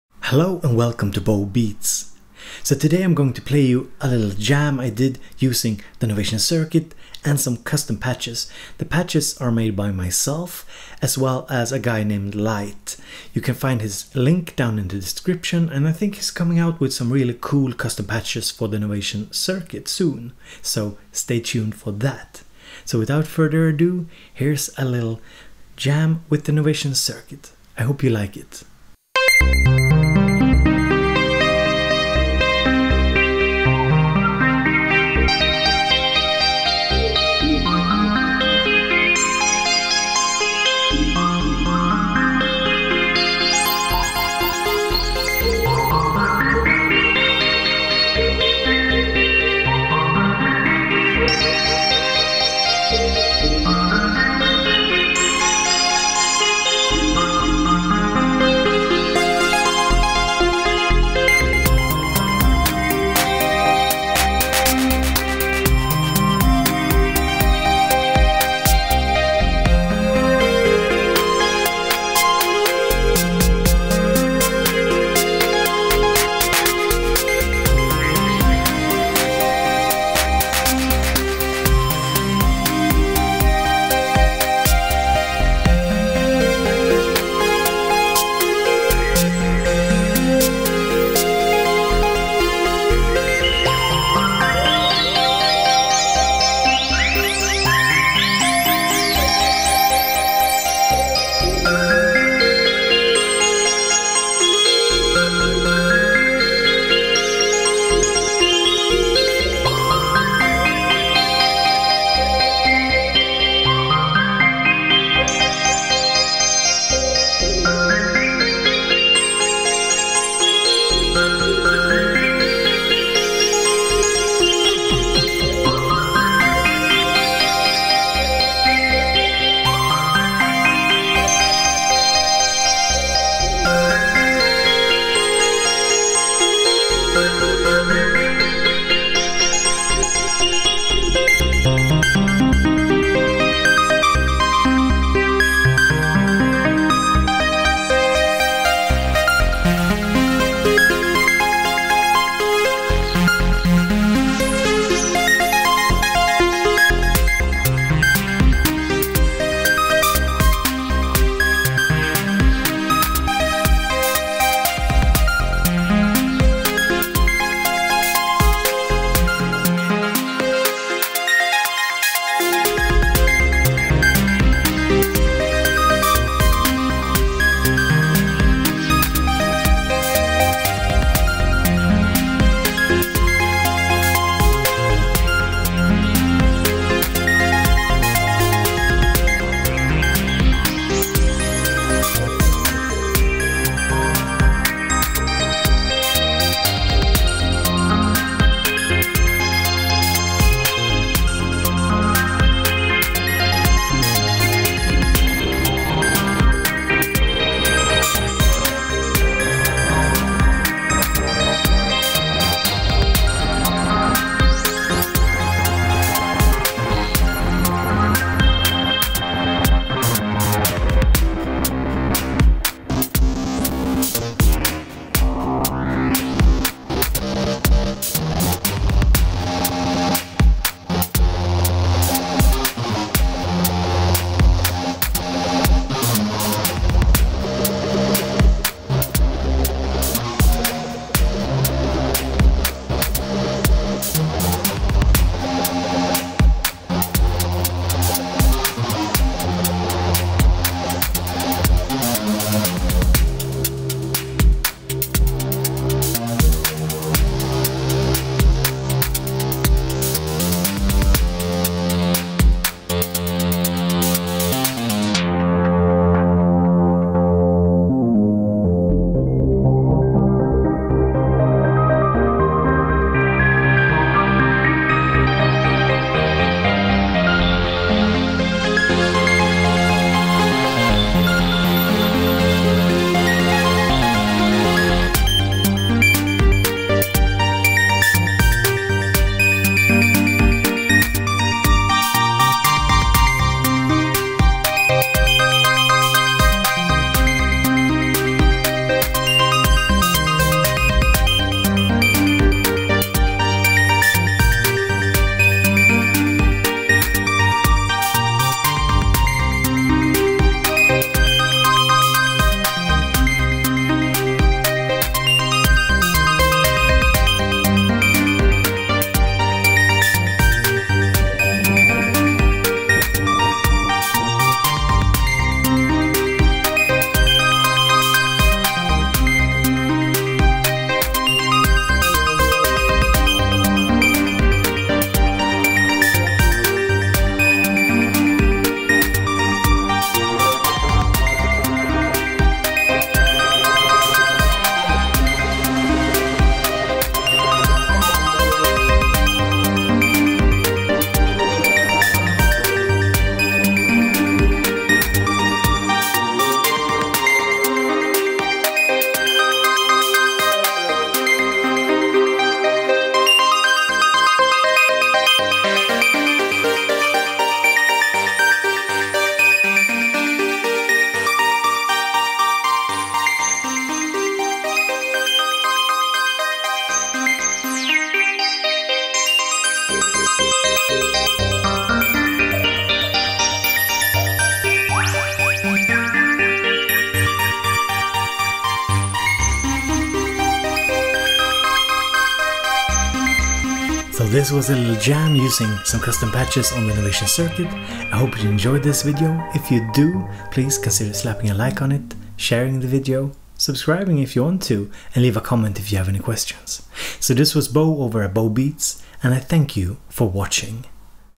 Is Circuit a Toy? - Circuit Jam with Custom Patches
This video is a Novation Circuit Jam. I tried to make an arpeggio sounding main lead that works both as a "bass" sound, arpeggio with lead elements. If anyone doubt the sonic potential of the Circuit this should put it to rest.